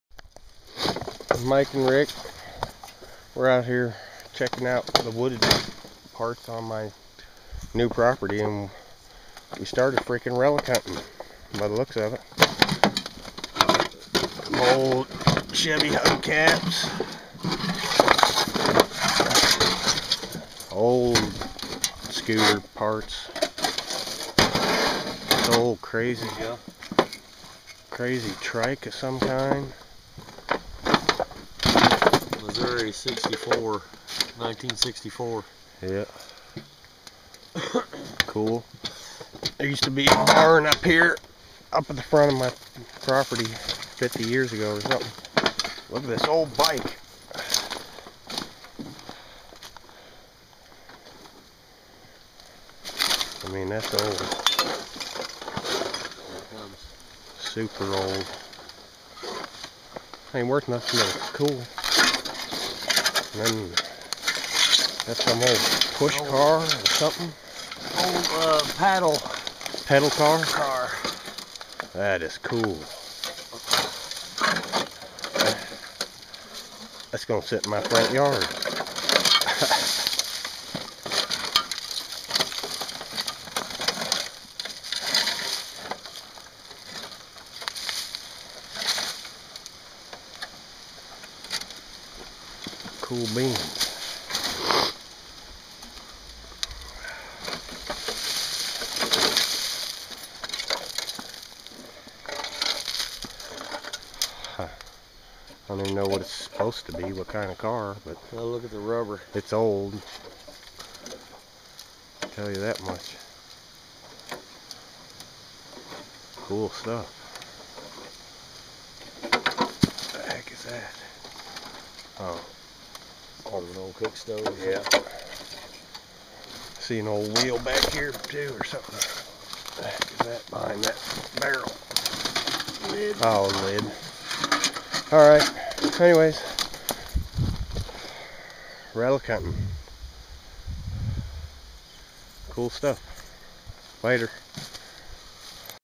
Relic huntin' at Ricks new place....
Mike and I checkin' out the woods on my new land. found some ool stuff!